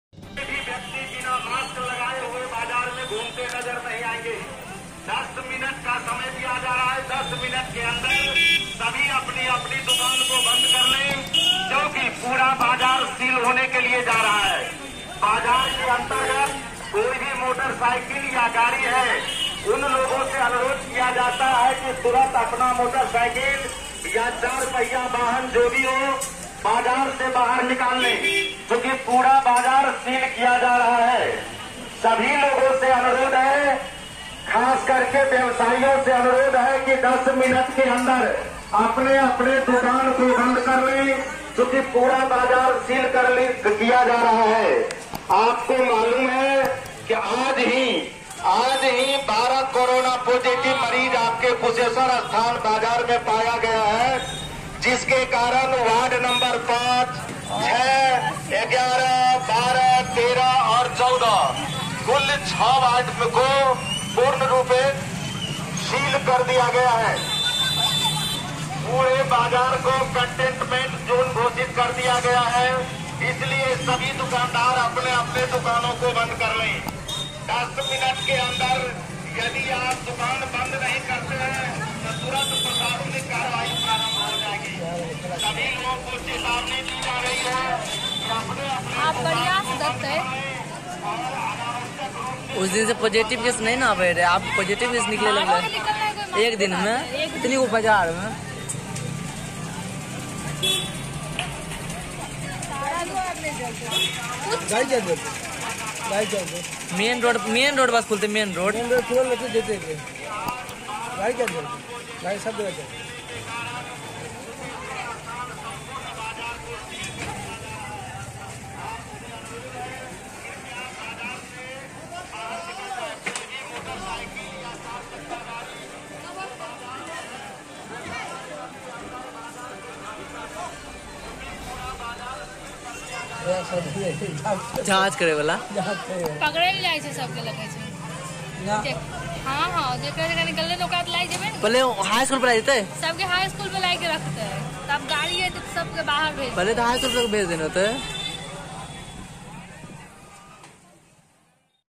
कुशेश्वरस्थान को कंटेनमेंट जोन घोषित करने के बाद पूर्णता बाजार को प्रशासन के द्वारा बंद करवाया जा रह
कुशेश्वरस्थान को कंटेनमेंट जोन घोषित करने के बाद पूर्णता बाजार को प्रशासन के द्वारा बंद करवाया जा रहा है